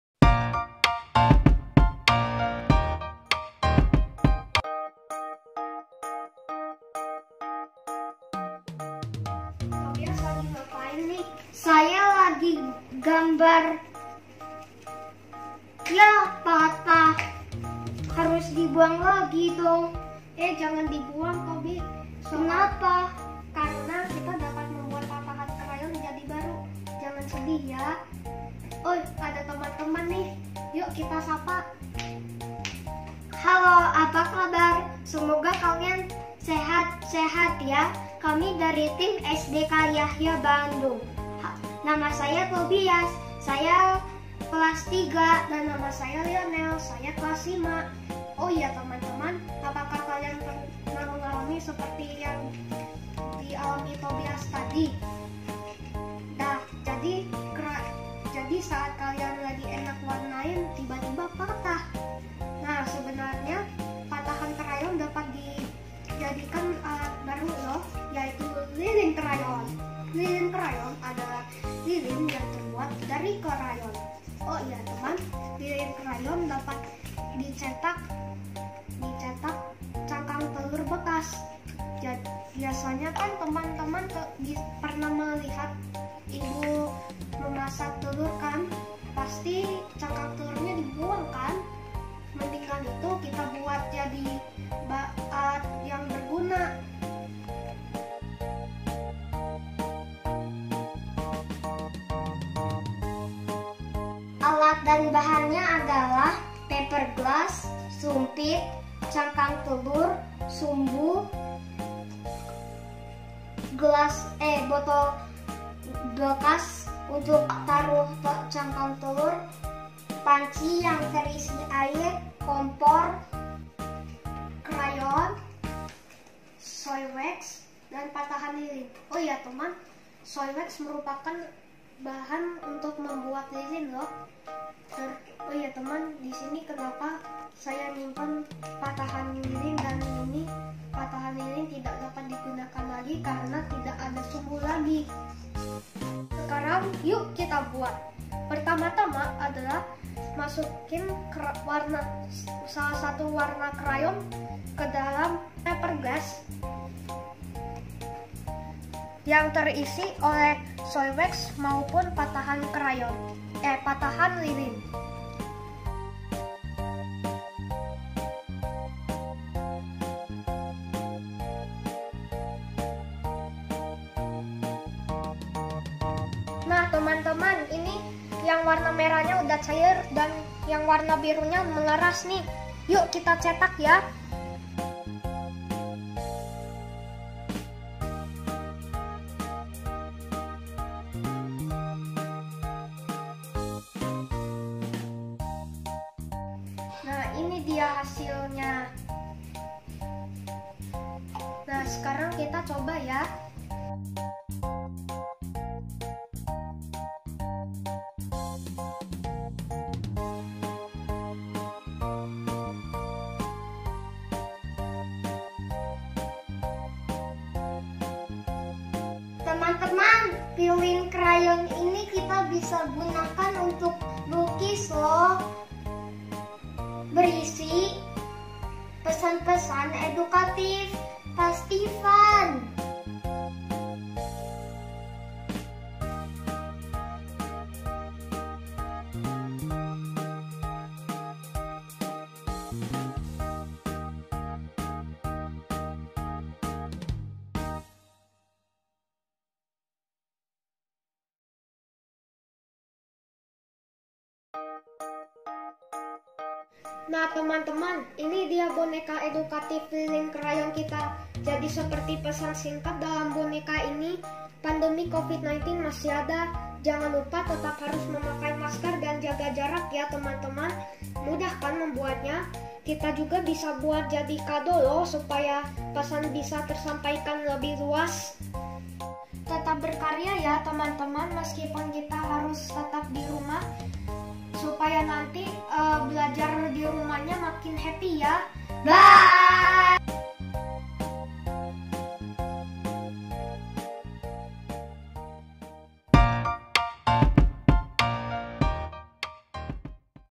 DKAI 2020 | SDK YAHYA | KATEGORI INOVASI | PILAR SENI & KREATIVITAS | CANDLE CRAYONS WITH EGG SHELLS
Nama Peserta: Lionel Isaac Mahardika Sitompul
                            Tobias Tamado Pradipta Sitompul
Umur : 11 tahun & 8 tahun
Asal Sekolah: SD Kristen Yahya
Kelas: 5 dan 3

Kategori lomba : Inovasi
Latar Belakang Video : ketika anak menggambar&mewarnai, sering crayon yang mereka gunakan patah. Dan itu menginspirasi mereka untuk mengalihfungsikan crayon tidak terpakai tersebut menjadi sesuatu yg berguna seperti lilin dan menggunakan cangkang telor bekas sebagai cetakannya. Untuk hiasan, mereka melukiskan pesan edukatif sehingga karya lukis yang hasil akhirnya berbentuk seperti boneka ini bisa berfungsi juga sebagai alat edukatif yang menarik bagi anak-anak.
Solusi dari ide/kreasi yg dibikin : Barang-barang anak-anak yg ada dirumah bisa dialihfungsikan menjadi alat edukatif yang menarik bagi anak2 bila barang tersebut sudah tidak terpakai lagi. Boneka edukatif yang dilukis pada cangkang lilin crayon menjadi salah satu solusi dari permasalahan crayon patah yg kerap dijumpai anak-anak.